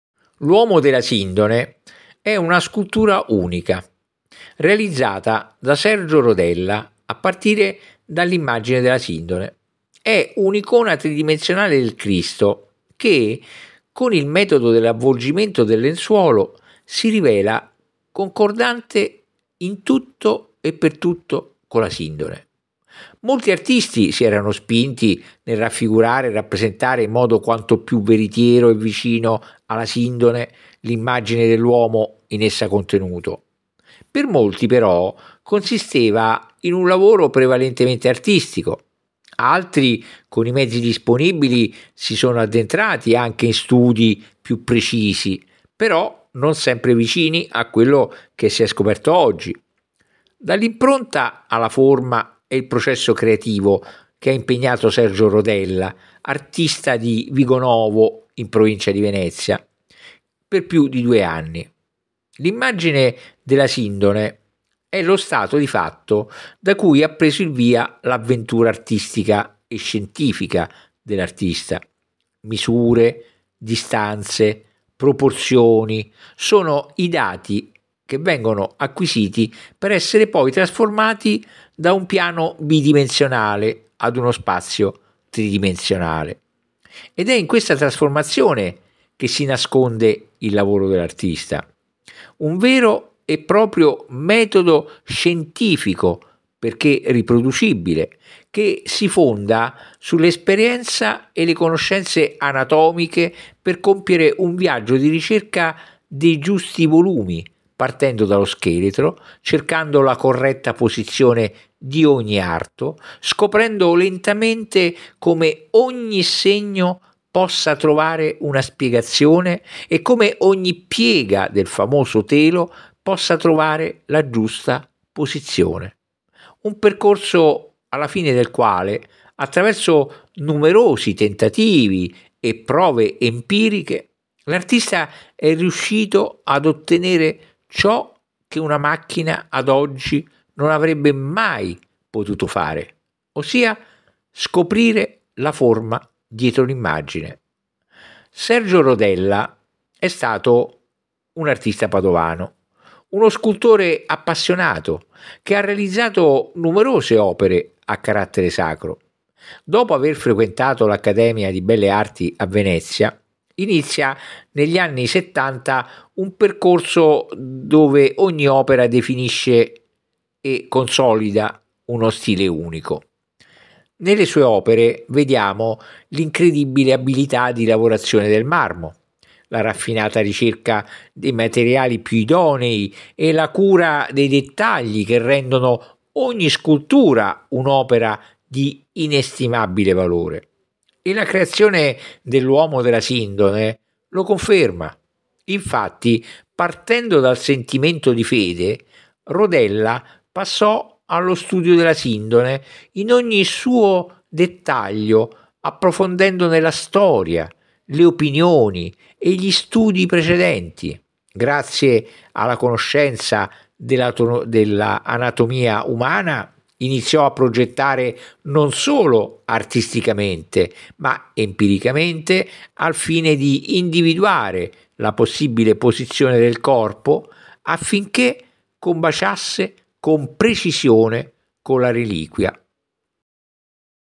Sergio Rondella e l'uomo della Sindone (Veneto) (creato con Spreaker)
L'uomo della Sindone è una scultura unica realizzata da Sergio Rodella a partire dall'immagine della Sindone.
È un'icona tridimensionale del Cristo che, con il metodo dell'avvolgimento del lenzuolo, si rivela concordante con la Sindone.
Dall'impronta alla forma è il processo creativo che ha impegnato Sergio Rodella, artista di Vigonovo in provincia di Venezia, per più di due anni.
L'immagine della Sindone è lo stato di fatto da cui ha preso il via l'avventura artistica e scientifica dell'artista.